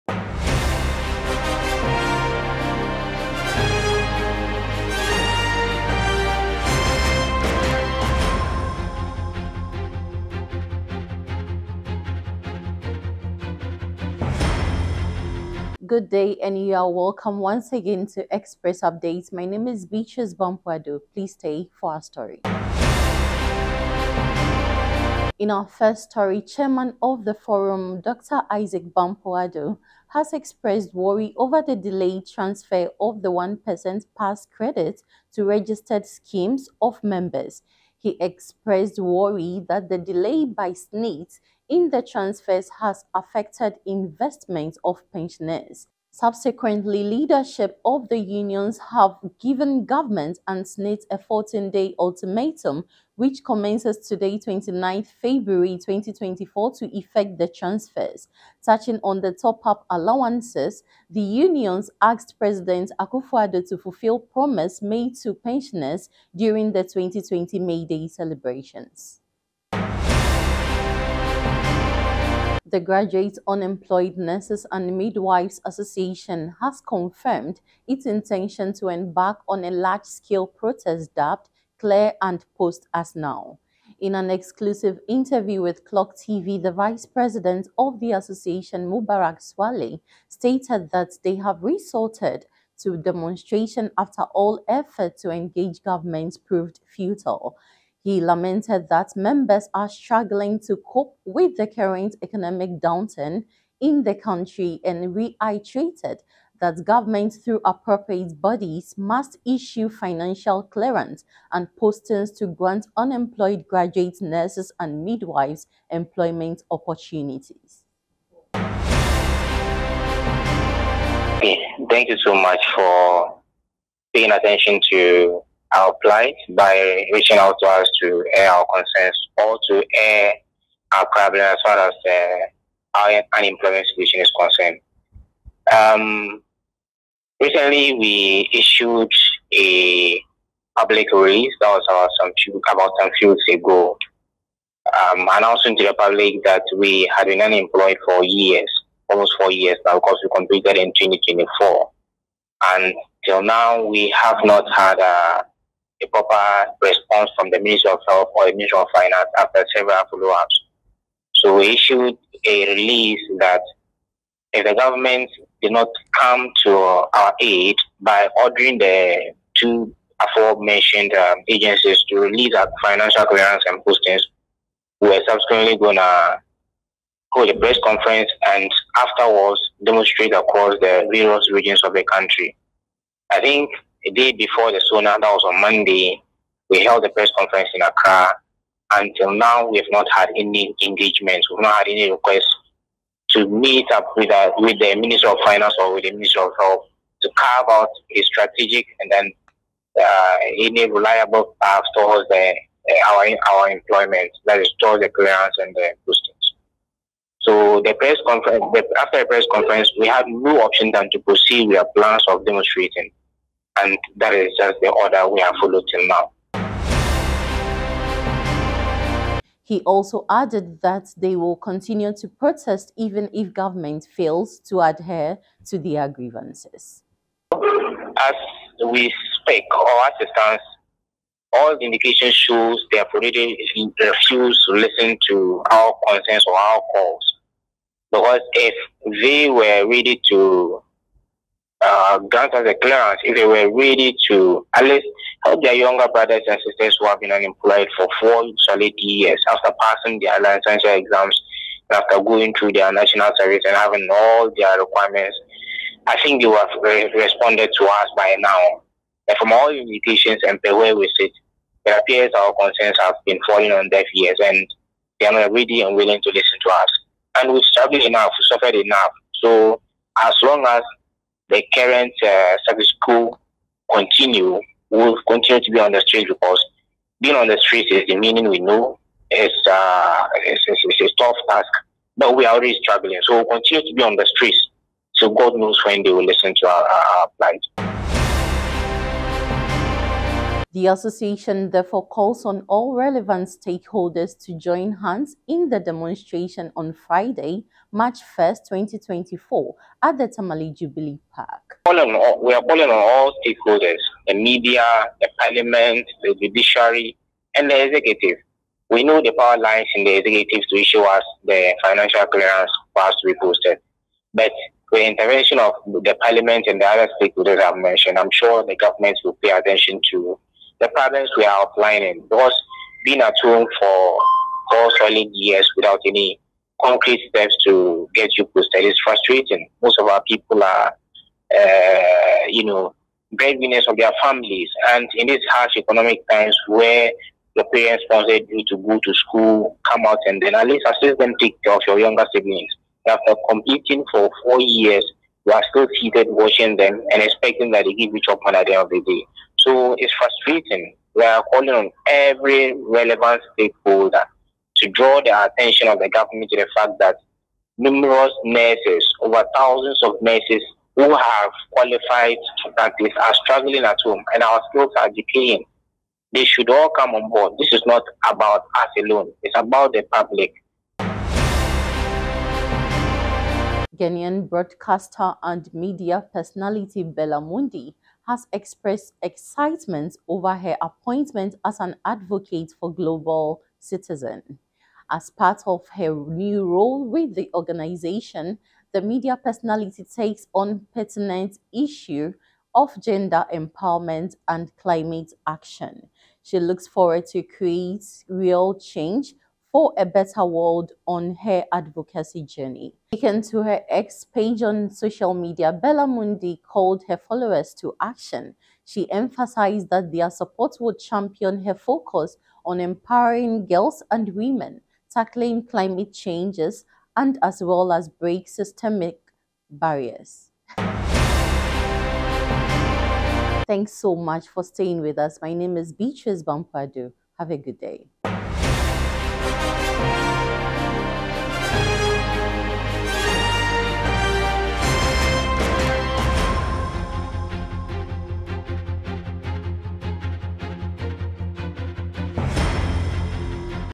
DELAY IN "PAST CREDIT" TRANSFERS AFFECT PENSIONERS INVESTMENT - FORUM
EXPRESS UPDATE | Delay in "past credit" transfers affect pensioners investment - Forum